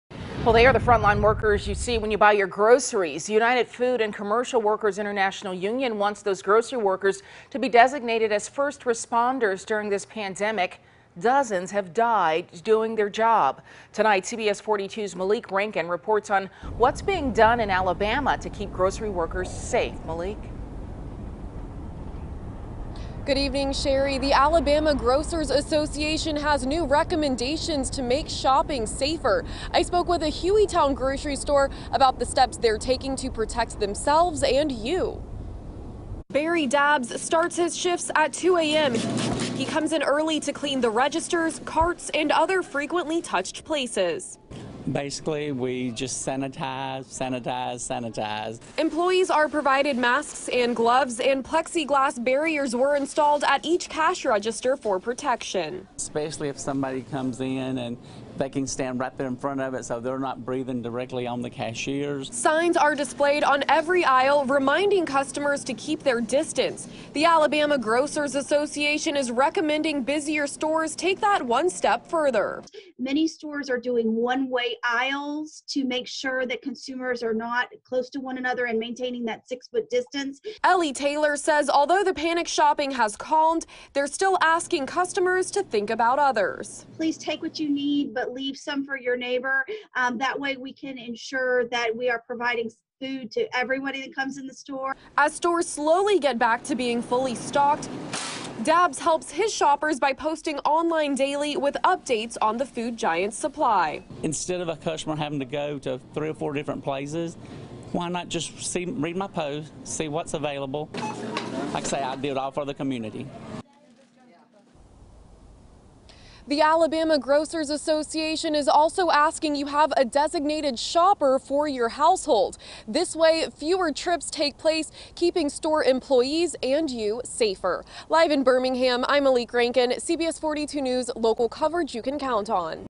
Grocery store safety recommendations from Alabama Grocers Association
According to the United Food and Commercial Workers International Union, more than 1,500 grocery store workers have tested positive for COVID-19.